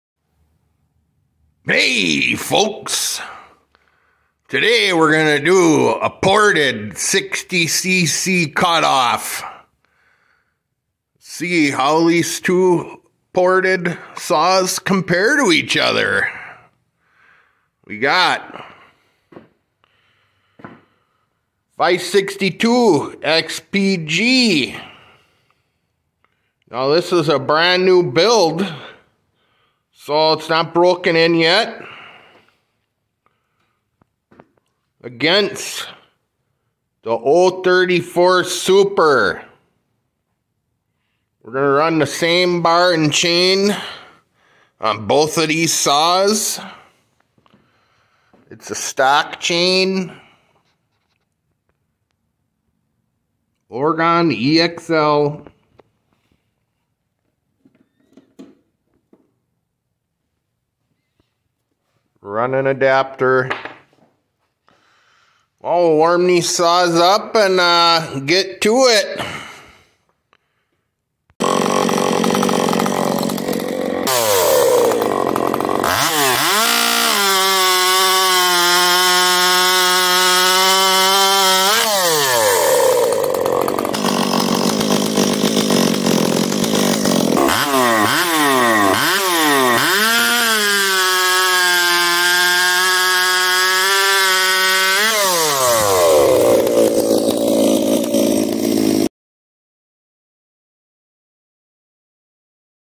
Ported 562 vs Ported 034 Super cut off!
Johnston Performance Chainsaw runs two ported 60cc saws against each other. New school AutoTune 562 against old school carburetor 034 super. This video was made entertainment purposes only.